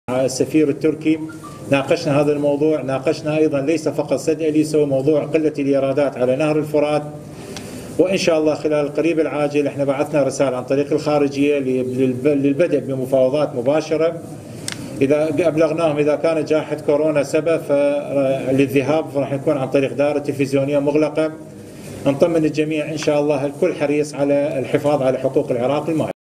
تصريح وزير الموارد المائية خلال المؤتمر الصحفي الذي عقد في أدارة موقع سد الموصل
تصريح السيد وزير الموارد المائية خلال المؤتمر الصحفي الذي عقد في أدارة  موقع سد الموصل حول المباحثات مع الجارة تركيا للحصول على حقوق العراق المائية